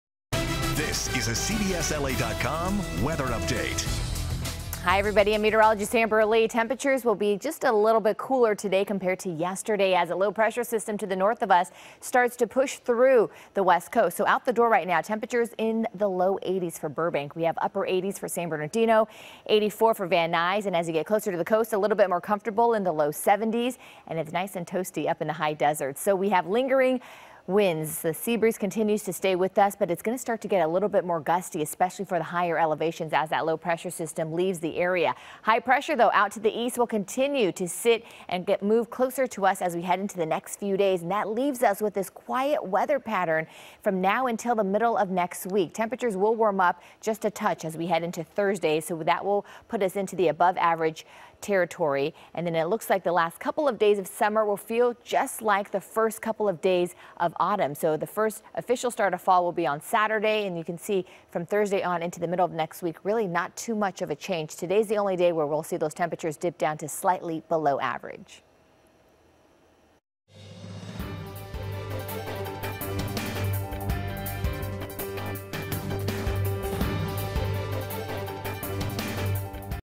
CBSLA Afternoon Weather Brief (September 19)
Amber Lee has your warm, dry forecast as we count down to the official end of summer and the beginning of fall.